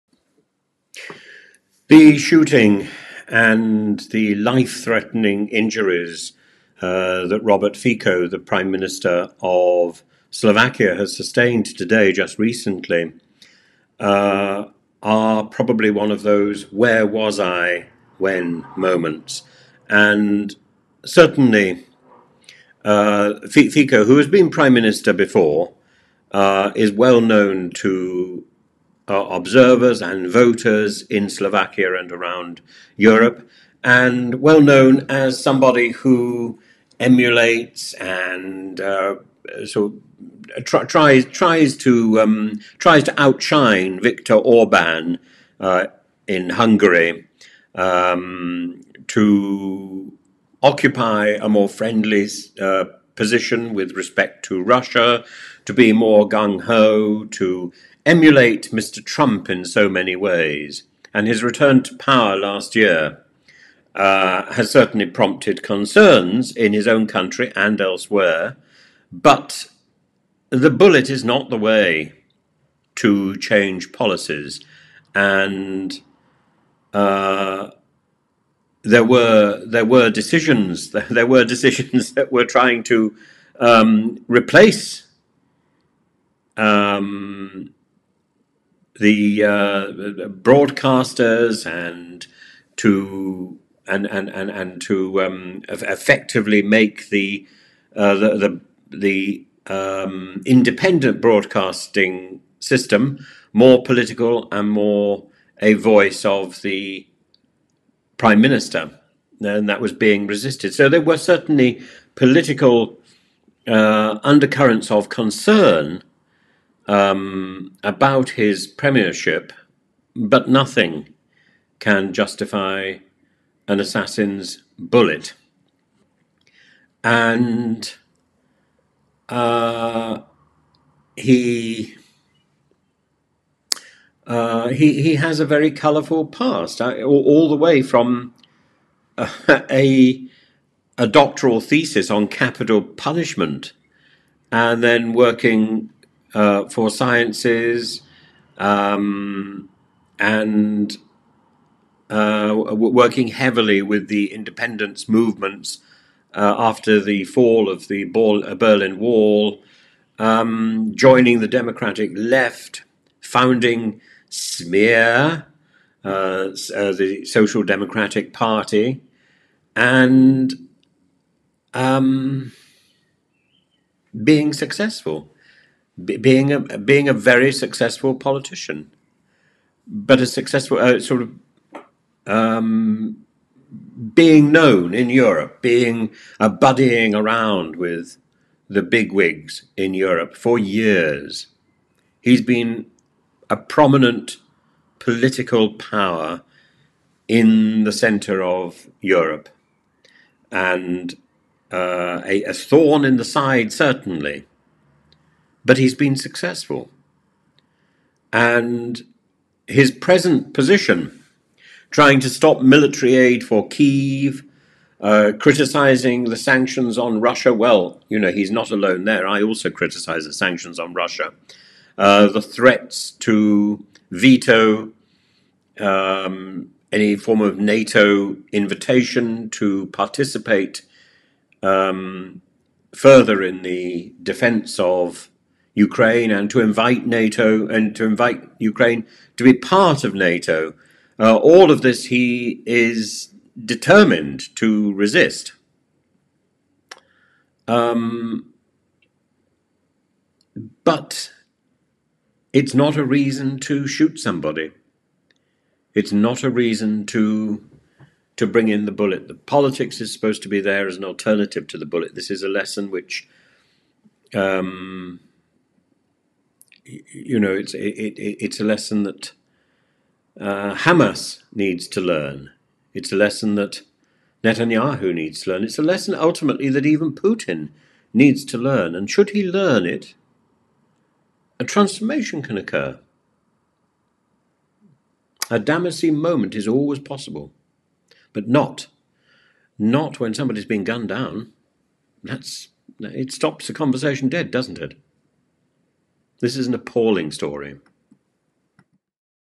the assassination attempt on the Slovak prime minister, Robert Fico
This is an appalling story and the fact that he pursues bombastic policies is no justification for the assassin's bullet